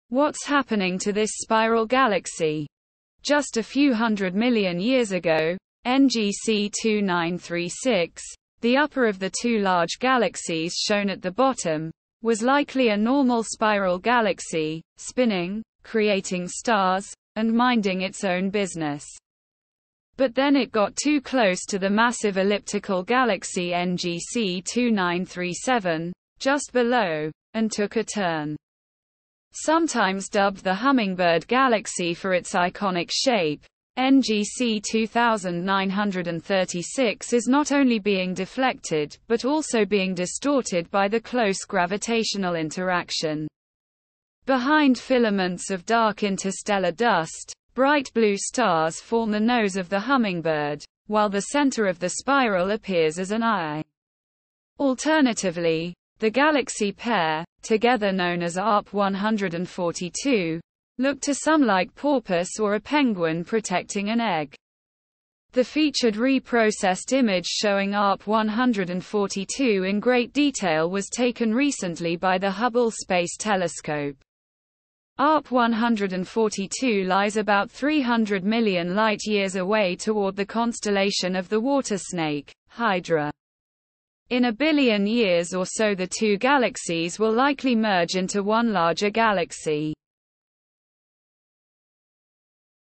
Arp 142: The Hummingbird Galaxy
What's happening to this spiral galaxy? Just a few hundred million years ago, NGC 2936, the upper of the two large galaxies shown at the bottom, was likely a normal spiral galaxy -- spinning, creating stars -- and minding its own business. But then it got too close to the massive elliptical galaxy NGC 2937, just below, and took a turn. Sometimes dubbed the Hummingbird Galaxy for its iconic shape, NGC 2936 is not only being deflected but also being distorted by the close gravitational interaction. Behind filaments of dark interstellar dust, bright blue stars form the nose of the hummingbird, while the center of the spiral appears as an eye.  Alternatively, the galaxy pair, together known as Arp 142, look to some like Porpoise or a penguin protecting an egg. The featured re-processed image showing Arp 142 in great detail was taken recently by the Hubble Space Telescope. Arp 142 lies about 300 million light years away toward the constellation of the Water Snake (Hydra). In a billion years or so the two galaxies will likely merge into one larger galaxy.